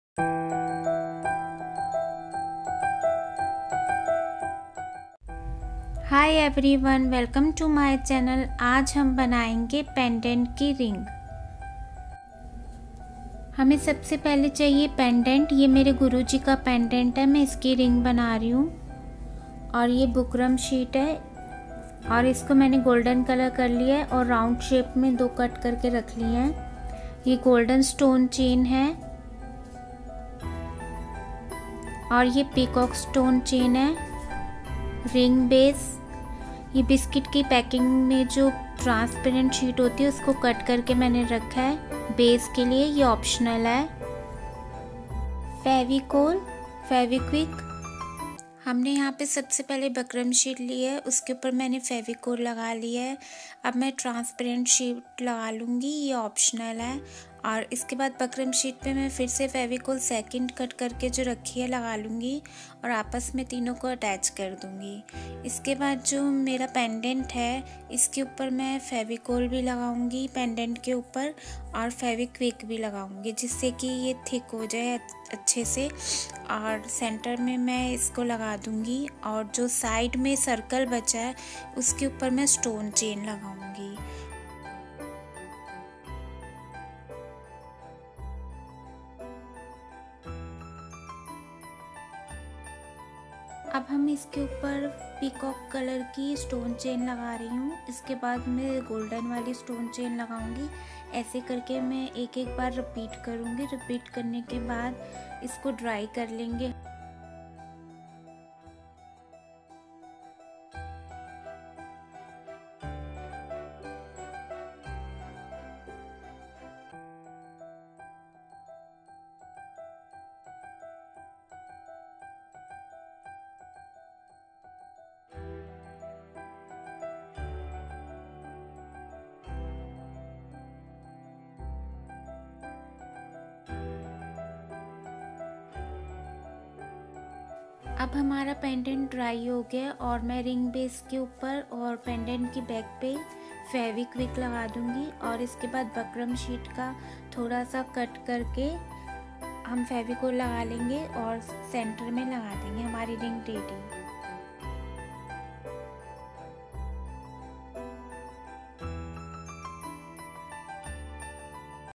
How to make Ring at home | Pendant se Ring banana sikhein | Diy Ring | Ring kaise banaye| Anghooti
In this video I will show you how to make ring using a pendant.

Ring diy,  how to make ring,  how to make jewellery, latest ring design,  kundan jewellery, polka jewellery,  pendant designs.

Jai Guruji | Wahe Guruji